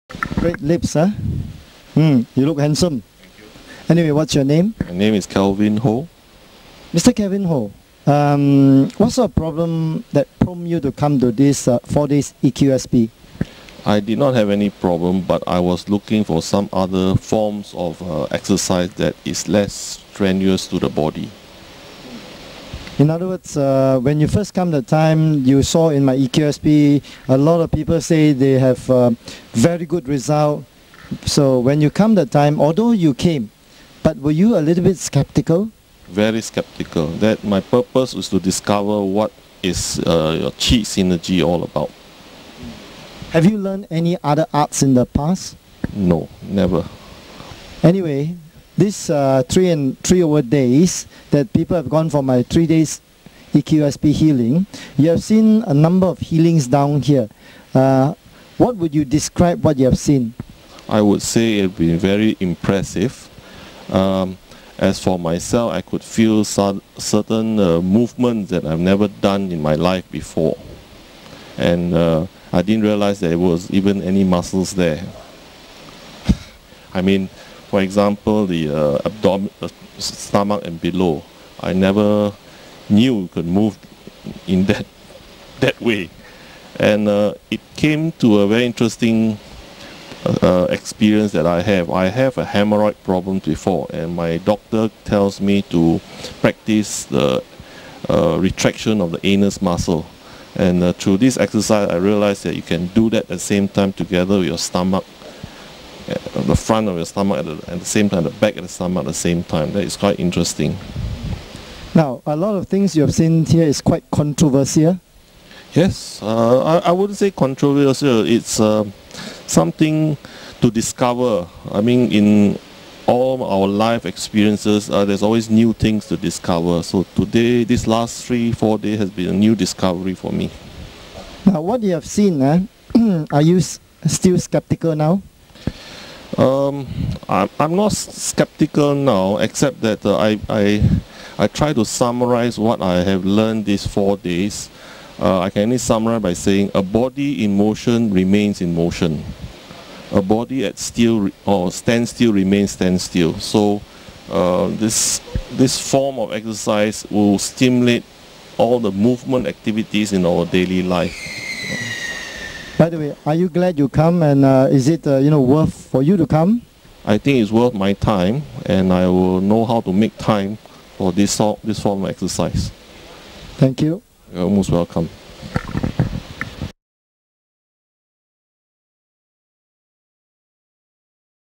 Miracle Hands Master Wong Resolves Haemmorhoid Problem
Mr Kelvin Ho Han Leong, Co. Director @50years old shares his benefits of our 4days EQSMP Wellness Seminar hosted by Master Wong

1) Health improvement quest -- have a haemorrhoid
     problem & also to improve myself on some form of
     exercise that is less strenuous to the body
     - Impressed with the EQSMP and worth to attend

2) Able to perform almost non-stop standing exercise from
    9am to 5pm except 2hours lunch & rest with occasional
    water replenishing and toilet calls & getting to be
    stronger as each day passes.  On 4th morning of
    EQSMP seminar, have lots of energy, feeling fresh to
    give voluntary testimony

3) Much Improvement in stamina, strength and health